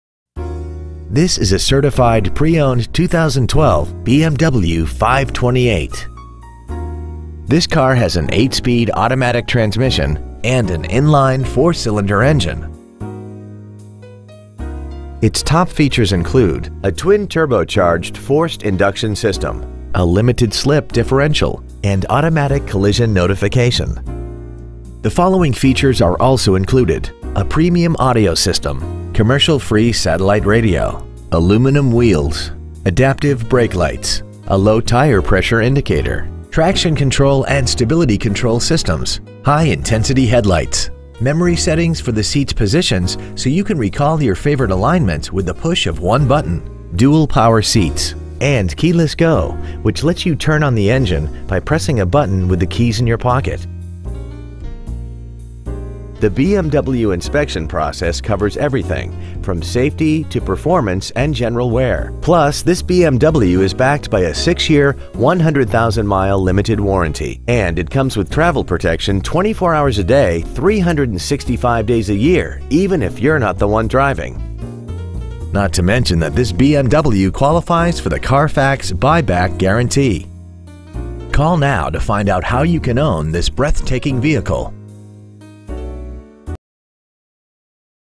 Pre-Owned 2012 BMW 528i The Woodlands TX 77384
Year : 2012
 Make : BMW
 Model : 528i
 Engine : Turbocharged Gas I4 2.0L/122
 Trans . : Automatic
 Exterior : Gray / Gray

 Interior : Gray